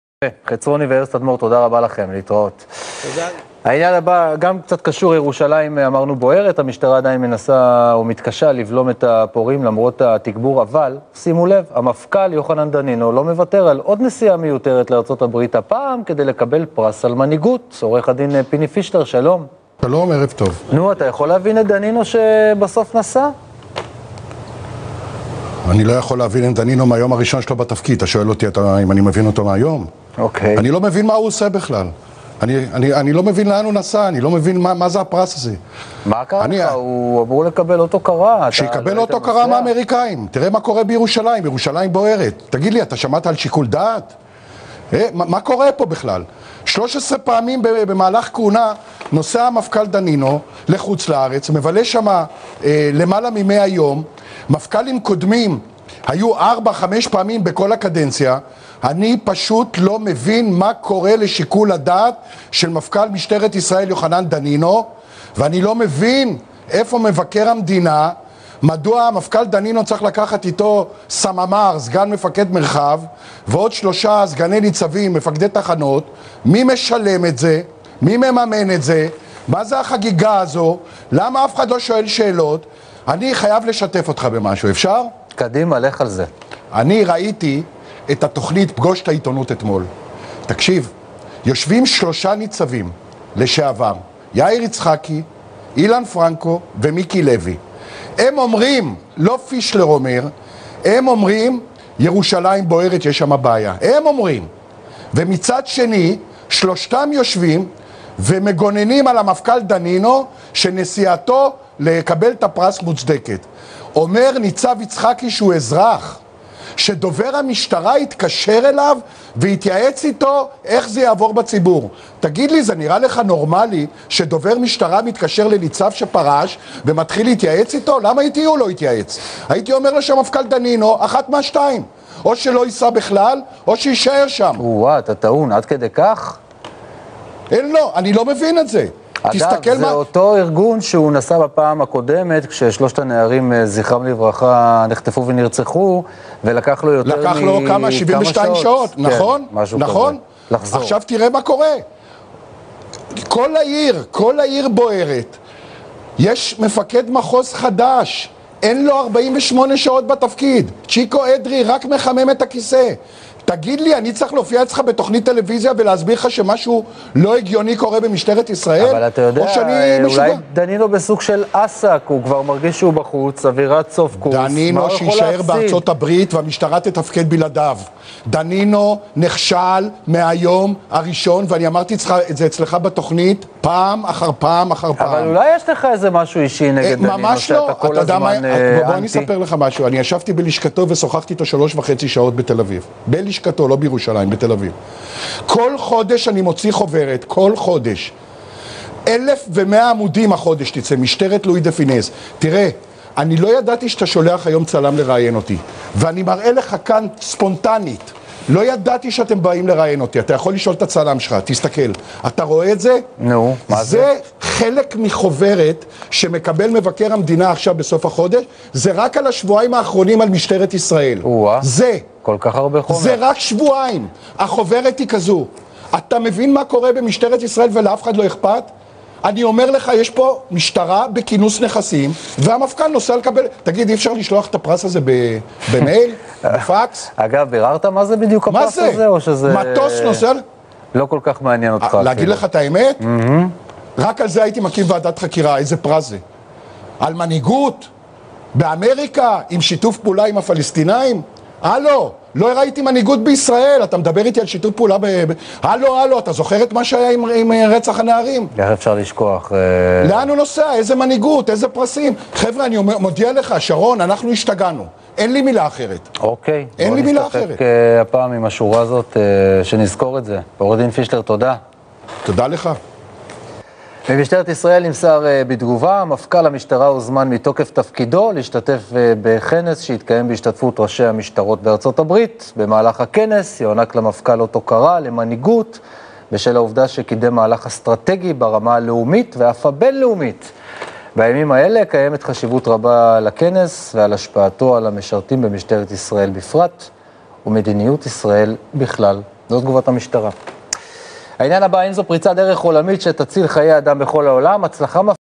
עו"ד פיני פישלר נגד מפכ"ל המשטרה יוחנן דנינו שלא מוותר על עוד נסיעה מיותרת לארה"ב
ערוץ 20, שרון גל
המפכ"ל יוחנן דנינו לא מוותר על עוד נסיעה מיותרת לארה"ב, הפעם כדי לקבל פרס על מנהיגות.
עו"ד פיני פישלר "אני לא מבין מה זה הפרס הזה". ירושלים בוערת. אין שיקול דעת.
13 פעמים במהלך כהונה נוסע המפכ"ל דנינו לחו"ל. מבלה שם למעלה מ- 100 יום. מפכ"לים קודמים היו 4 - 5 פעמים בחו"ל בכל הקדנציה שלהם. אני פשוט לא מבין מה קורה לשיקול הדעת של מפכ"ל משטרת ישראל יוחנן דנינו  ואני לא מבין איפה מבקר המדינה. מדוע המפכ"ל דנינו צריך לקחת איתו סממ"ר סגן מפקד מרחב, ועוד שלושה סגני ניצבים מפקדי תחנות. מי משלם את זה? מי מממן את זה? מה זה החגיגה הזו? למה אף אחד לא שואל שאלות?!
אני ראיתי את התוכנית "פגוש את העיתונות" אתמול, יושבים שלושה ניצבים לשעבר: יאיר יצחקי, אילן פרנקו ומיקי לוי. הם אומרים "ירושלים בוערת יש שם בעיה", ומצד שני שלושתם יושבים ומגוננים על המפכ"ל דנינו שנסיעתו לקבל את הפרס מוצדקת.
אומר ניצב יצחקי שהוא אזרח, שדובר המשטרה התקשר אליו והתייעץ איתו איך זה יעבור בציבור. זה נראה לך נורמלי שדובר משטרה מתקשר לניצב שפרש ומתחיל להתייעץ איתו. למה איתי הוא לא התייעץ? הייתי אומר לו שהמפכ"ל דנינו אחת מהשניים: או שלא יסע בכלל או שיישאר שם.
כל העיר ירושלים בוערת. יש מפקד מחוז חדש אין לו 48 שעות בתפקיד. צ'יקו אדרי רק מחמם את הכיסא.
דנינו נכשל מהיום הראשון. יש פה משטרה בכינוס נכסים ולאף אחד לא איכפת. רק על הפרס הזה הייתי מקים ועדת חקירה.